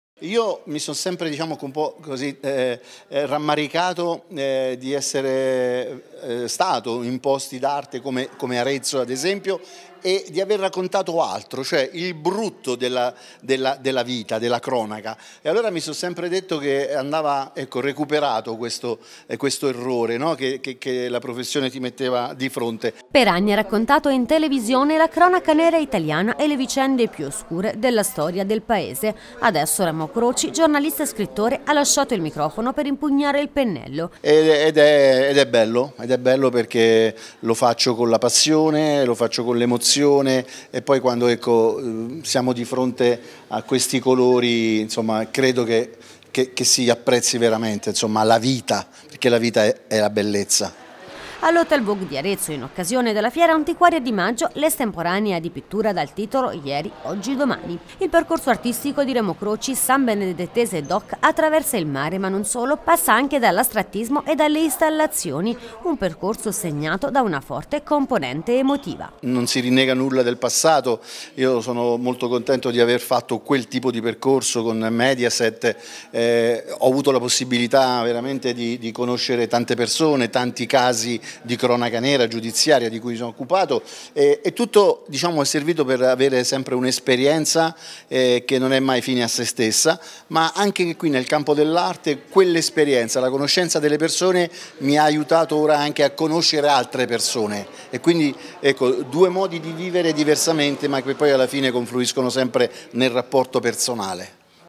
«Ieri Oggi Domani», la mostra di Remo Croci ad Arezzo
Per anni ha raccontato in televisione la cronaca nera e le vicende più oscure della storia italiana.
Adesso Remo Croci, giornalista e scrittore, ha lasciato il microfono per impugnare il pennello e dar colore.
L'hotel Vogue di Arezzo, in occasione della Fiera Antiquaria di maggio, ha ospitato l'estemporanea di pittura dal titolo «Ieri Oggi Domani».